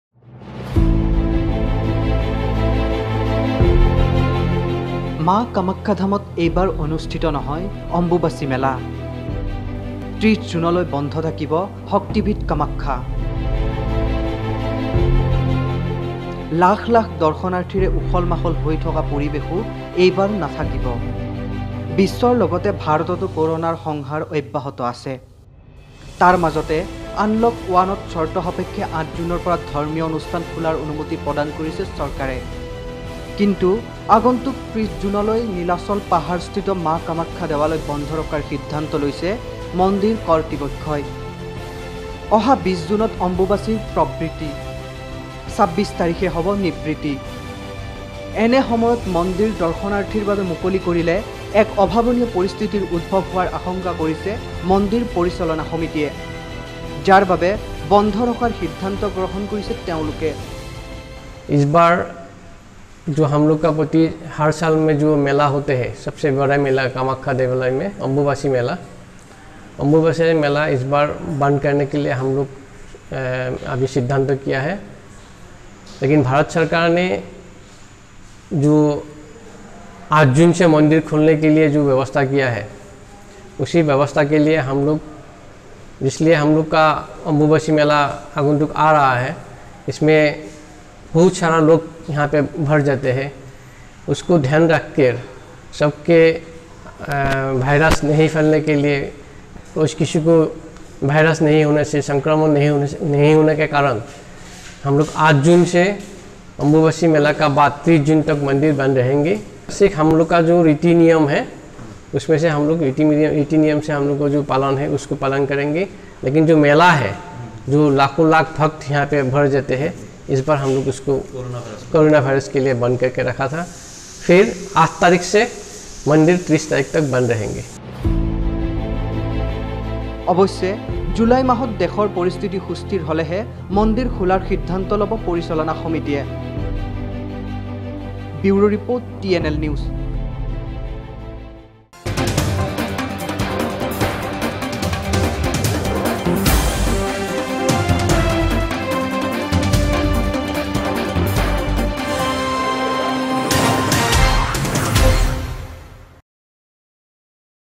এইবাৰ কামাখ্যাত নহয় অম্বুবাচী মেলা ৩০ জুনলৈ বন্ধ শক্তিপীঠ কামাখ্যা
এইবাৰ কামাখ্যাত নহয় অম্বুবাচী মেলা
৩০ জুনলৈ বন্ধ শক্তিপীঠ কামাখ্যা